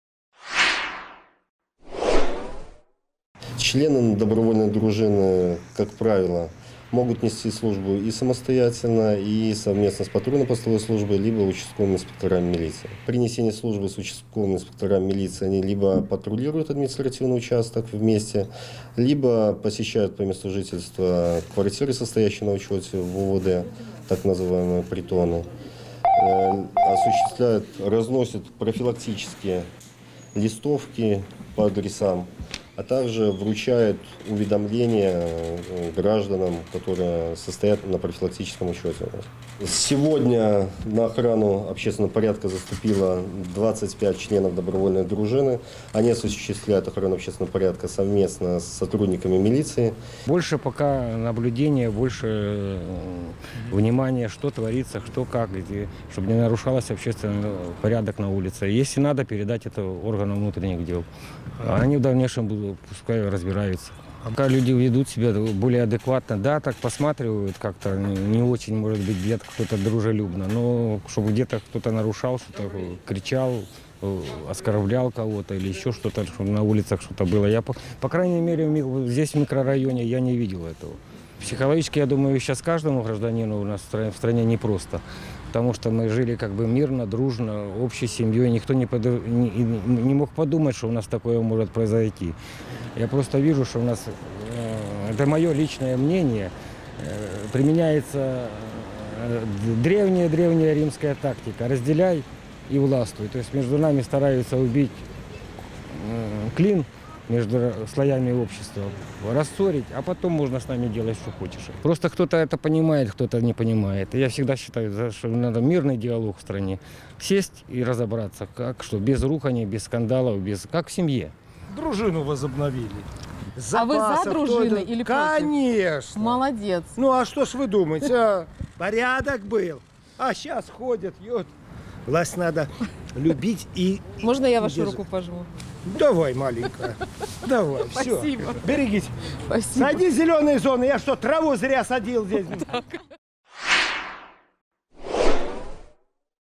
Добровольные дружины помогают милиции
Добровольные дружины помогают милиции в охране правопорядка.